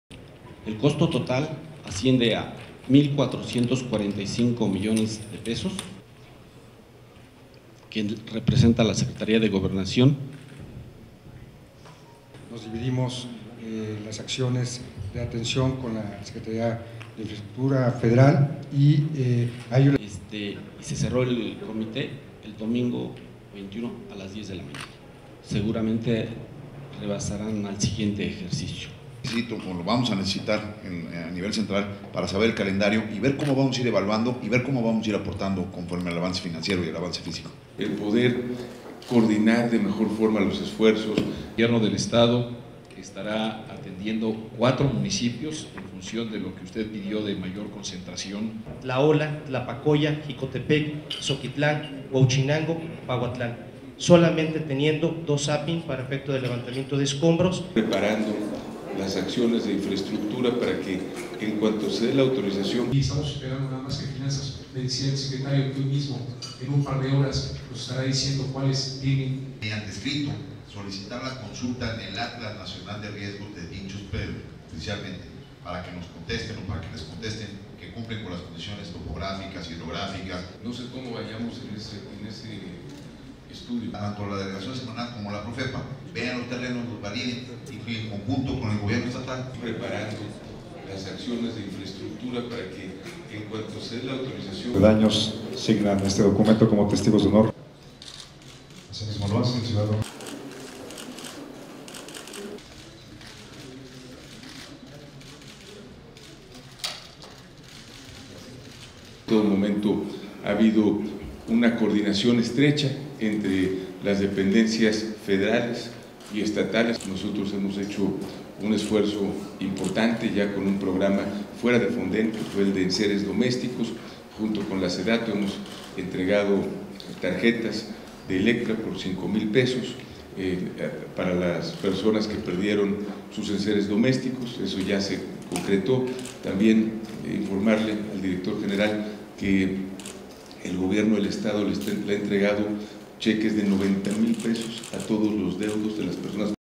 SE FIRMA EL ACTA DE ENTREGA DE RESULTADOS DEL COMITÉ DE EVALUACIÓN DE DAÑOS DEL FONDEN
-      El gobierno estatal continuará en estrecha coordinación con la federación para iniciar la etapa de reconstrucción en todos los municipios declarados en desastre, afirmó el gobernador Rafael Moreno Valle.
-      Durante la sesión de entrega de resultados, informó que su administración ya cuenta con el recurso para atender las afectaciones.

A dicha  entrega de resultados del Comité de Evaluación de Daños del Fondo de Desastres Naturales (Fonden), asistió el director general para la Gestión de Riesgos, José María Tapia Franco.
El funcionario apuntó que el apoyo del gobernador permitió que la labor sea pronta y eficaz, para atender en menor tiempo posible la cuantificación de los daños y contar con el recurso para iniciar la reconstrucción.
Moreno Valle dijo que para realizar la reparación ágilmente, se distribuyeron las acciones que atenderá cada instancia.
En la sesión, el secretario de Finanzas y Administración, Guillermo Bernal Miranda, dio a conocer que en un diagnóstico preliminar, la evaluación total de daños asciende a...